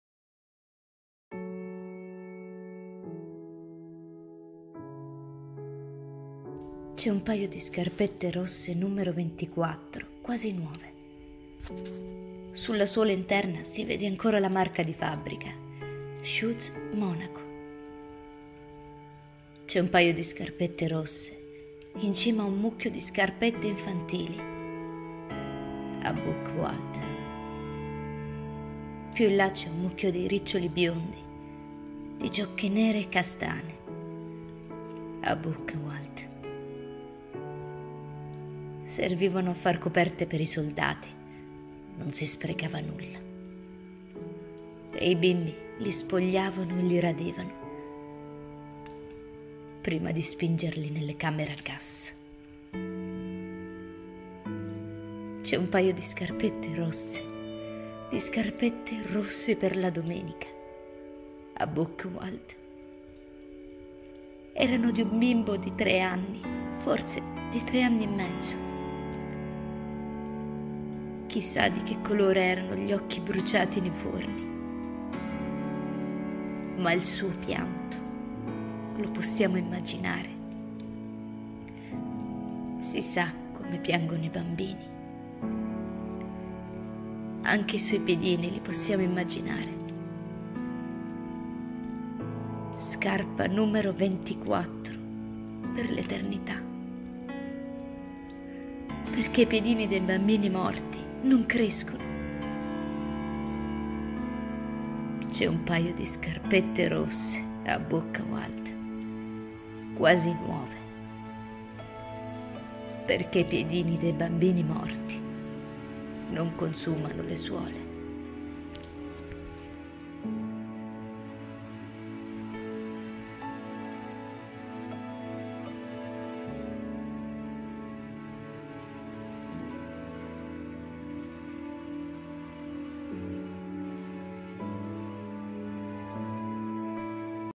C'è un paio di scarpette rosse" di Joyce Lussu. Interpretato da Rosemary Calderone
Per la giornata della memoria dedicato alle vittime dell'Olocausto
"c'è  un paio di scarpette rosse" poesia di Joyce Lussu
interpretato da
Rosemary Calderone